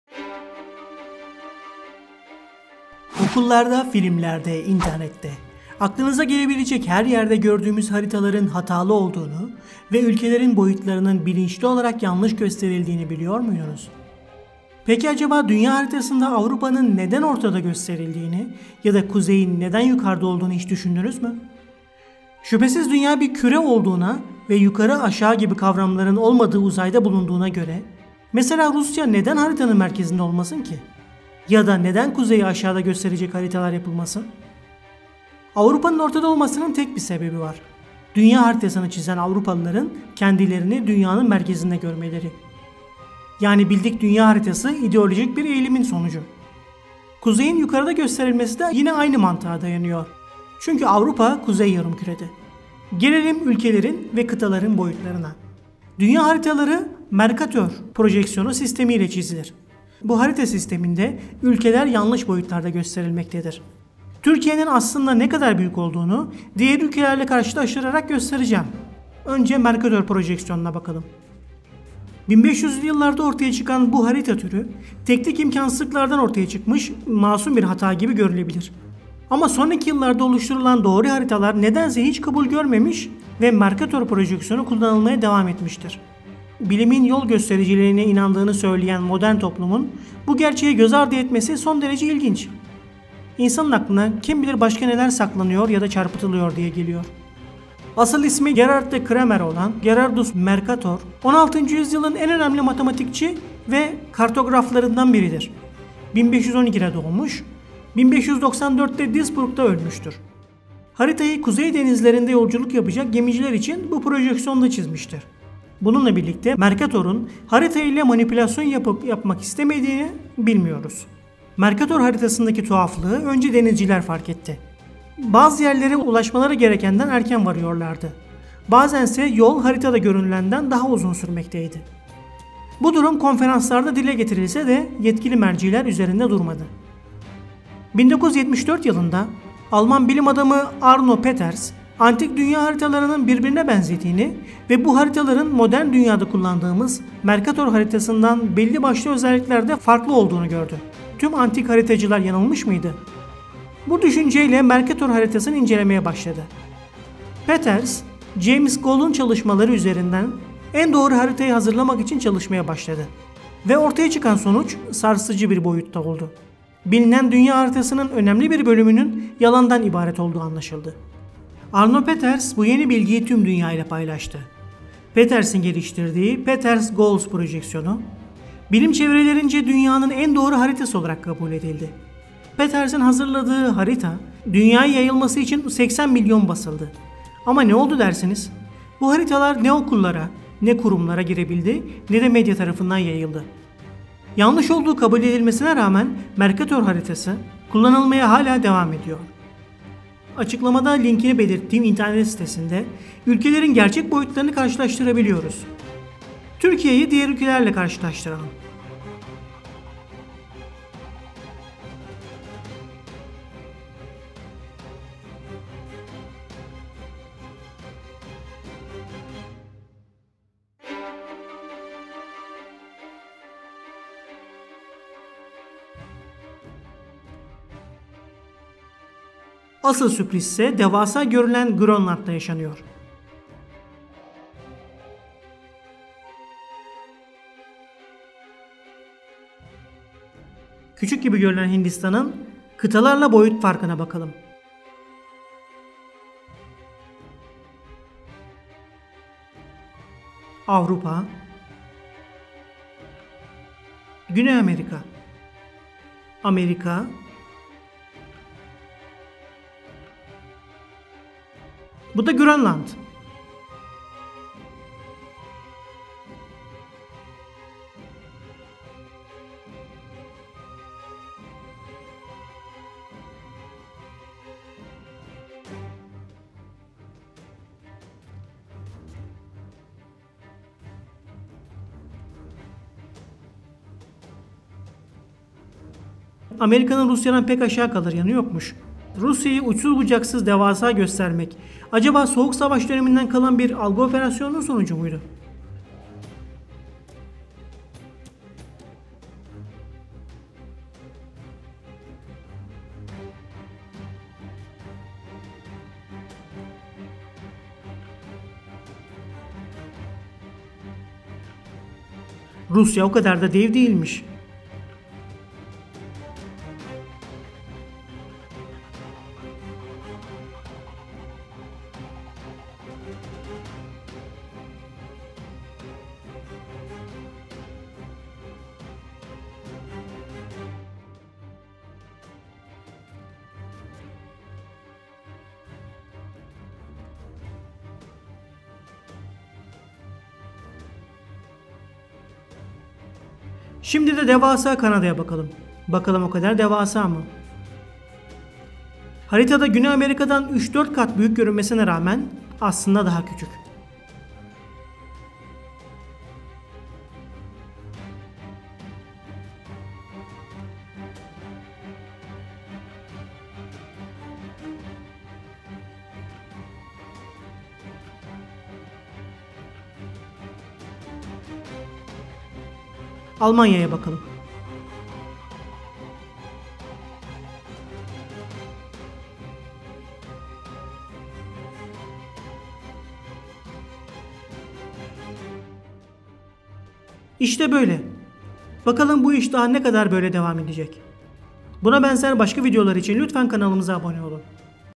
Haritalarda Türkiye'yi Küçük Gösteriyorlar. Hayatımız Boyunca Kandırıldık.
Küre şeklindeki dünyamız üzerindeki ülkeler haritalarda gösterilirken ister istmez deformasyona uğruyor ve birebir aktarılamıyor. Ne var ki bazen boyutlar öyle çok değişiyor ki bunların iyi niyetli değiştirmeler olduğuna inanmak zor. Bu videoda olduğundan daha büyük ya da küçük gösterilen ülkelere, kıtalara göz atacağız ve gerçek boyutlarını göstermeye çalışacağız. Herşey Meraktan ekibi keyifli seyirler diler. (Gerçi iki kişiyiz ama ekip sayılır)